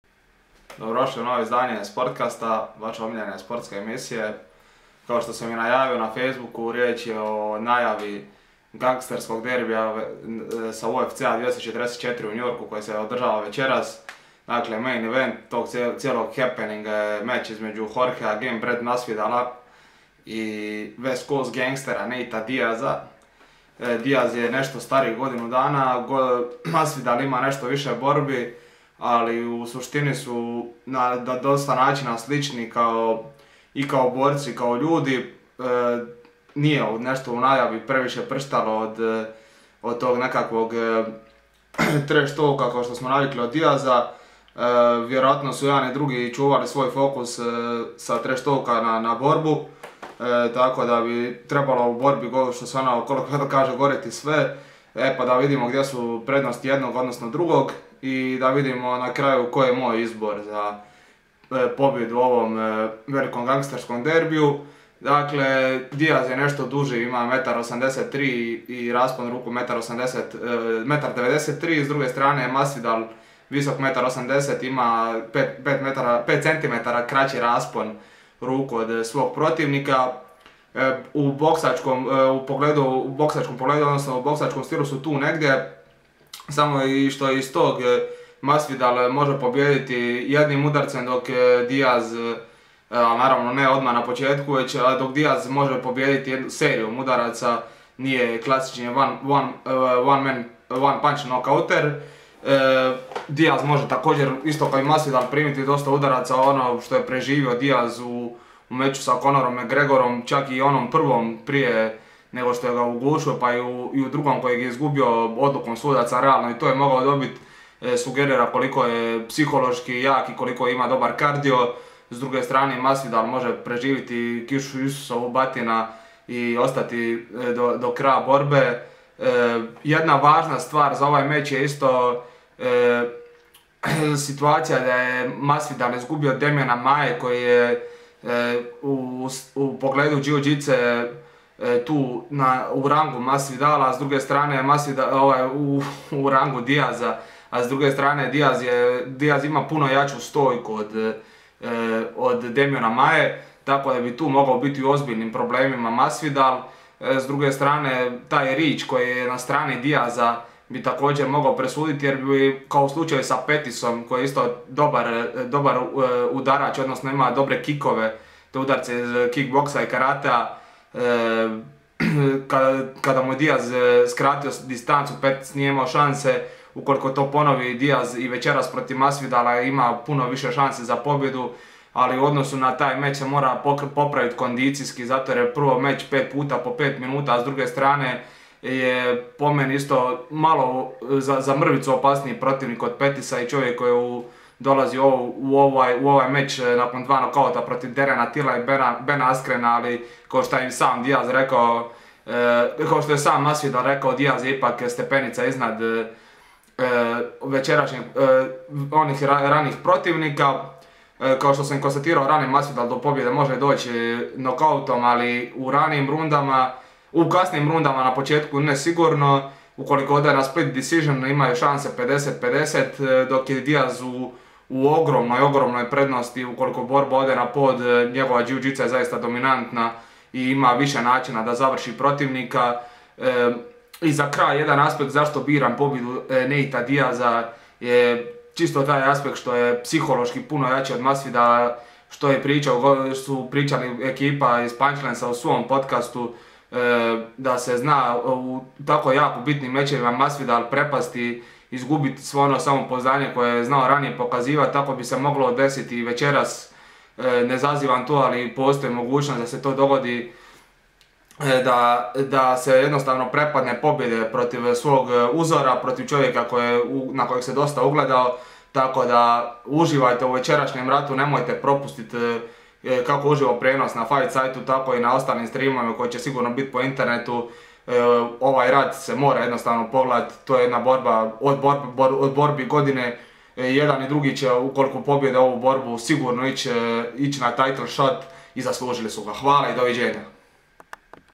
Najava borbe za BMFBELT- Nate Diaz vs Jorge Masvidal-- (28.epizoda)
U novoj epizodi Sportcasta bavim se najavom "Gangsterskog derbija" između "West Coast" I "East Coast" Gangstera"